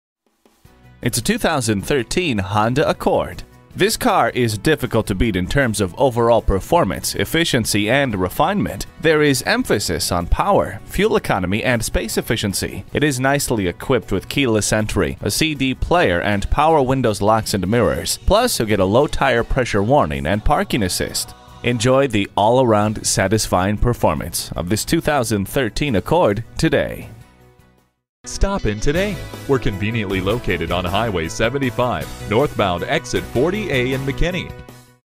2013 Honda Accord Sdn Dallas TX McKinney, TX DA202742 SOLD
Year: 2013
Make: Honda
Model: Accord Sdn
Trim: 4dr I4 CVT EX-L Sedan
Engine: 2.4L DOHC MPFI 16-VALVE I
Transmission: Automatic
Color: White Orchid Pearl
Interior: Ivory
Stock #: DA202742
This 2013 Honda Accord Sdn 4dr 4dr I4 CVT EX-L Sedan features a 2.4L DOHC MPFI 16-VALVE I 4cyl Gasoline engine. It is equipped with a Automatic transmission. The vehicle is White Orchid Pearl with a Ivory interior. It is offered with a full factory warranty.

Address:
601 S Central Expy